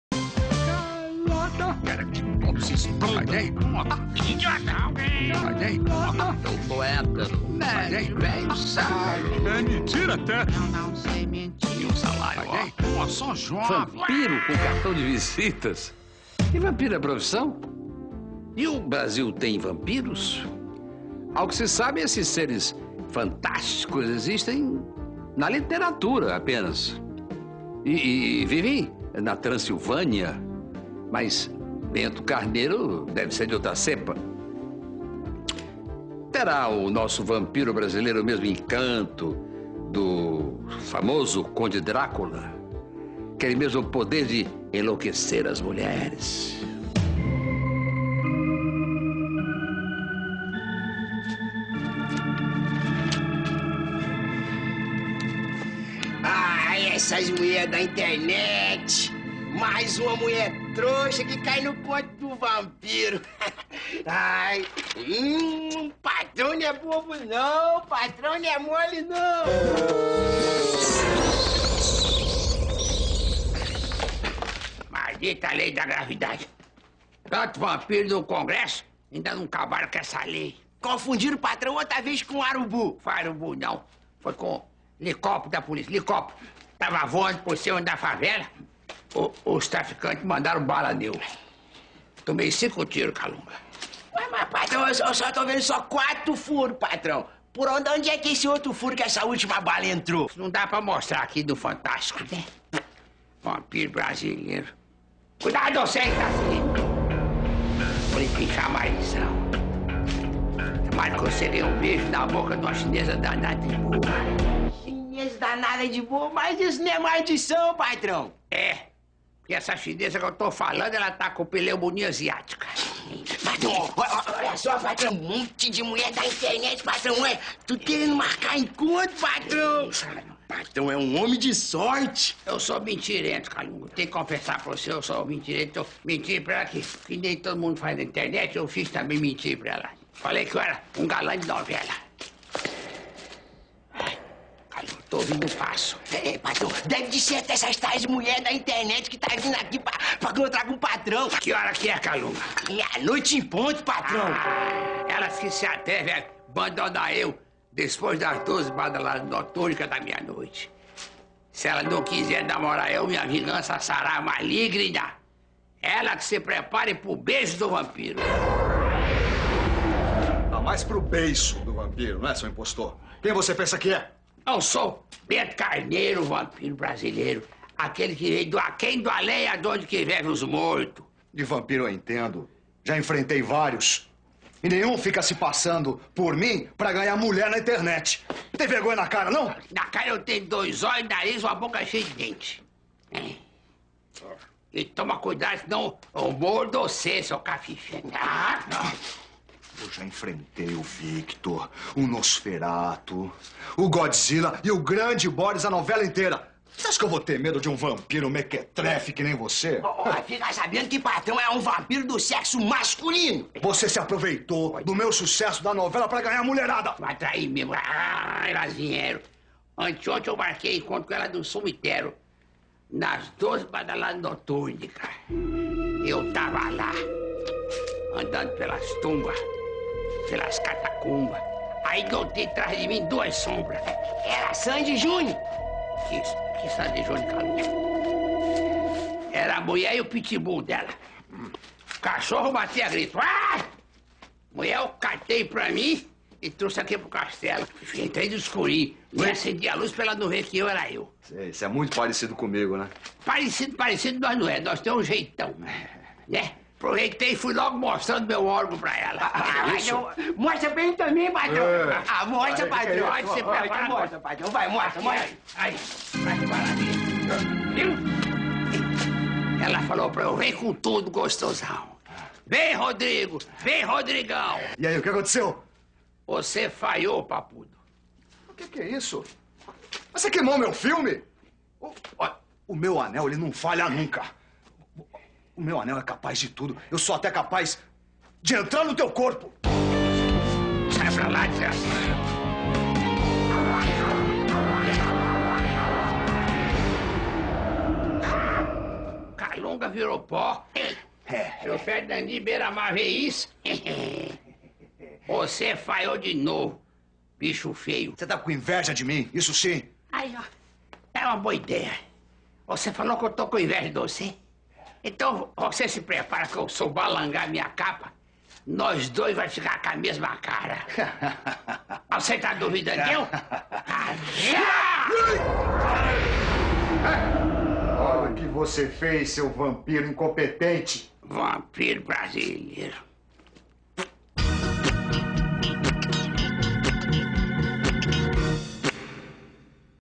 Chico Anysio fala do vampiro Bento Carneiro que tenta atrair mulheres pela internet.
24/03/2012 02h56- Atualizado em 24/03/2012 02h58
VÍDEOS: Chico Anysio.
Humorista morreu nesta sexta-feira (23), aos 80 anos.
Veja imagens dos personagens e entrevistas do comediante.

Chico Anysio fala do vampiro Bento Carneiro. Depois do depoimento do humorista, é exibido o quadro em que Carneiro tenta atrair mulheres pela internet.
Crédito:  G1